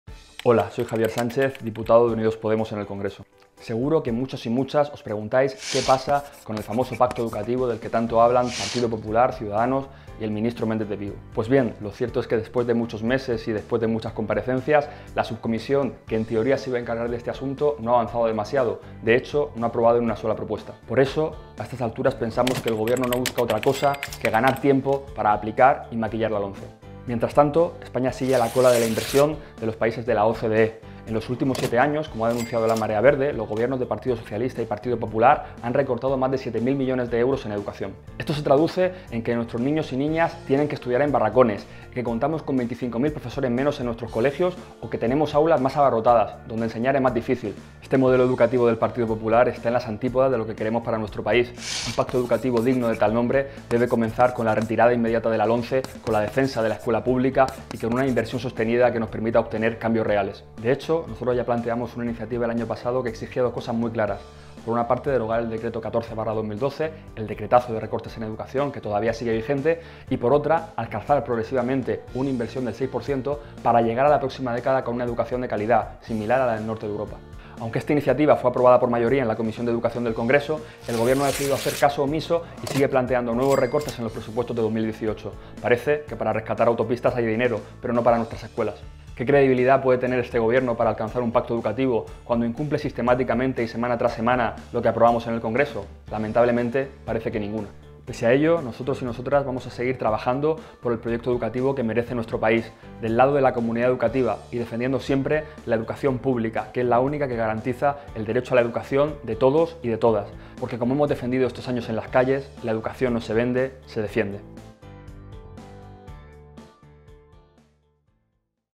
¿Qué pasa con el Pacto de Estado por la Educación? Javier Sánchez.
En este vídeo, nuestro diputado Javier Sánchez, nos explica cómo el Partido Popular está utilizando la subcomisión del Pacto de Estado por la Educación como pretexto para seguir aplicando la LOMCE y más recortes.